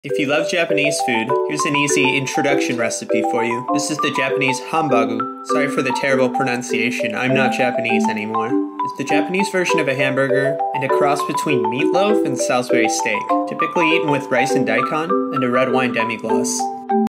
Easy Japanese Hambagu
How to make Japanese style hamburgers (hambagu) (:

Recipe (for 4-5):
1c ground beef
1c ground pork
1 whole onion (diced and sauted)
2 eggs
2tsp salt
1/2tsp pepper
2tbs panko breadcrumbs
3tbs milk

- Mix together and form into oval shapes

Sauce Recipe:
1/2c red wine
4tbs water

1/2c +1tbs ketchup
3tbs worcestershire
2tbs sugar
1tbs dijon mustard

- Add the red wine and water when your flip your hambagu and steam for 5 minutes.
- Take out the hambagu and add the rest of the ingredients.
- Cook over medium until it forms a pourable sauce (almost like bbq sauce)
- Pour over your hambagu and enjoy with rice, daikon and other vegetables!

My Equipment (:
Chef Knife
Sushi knife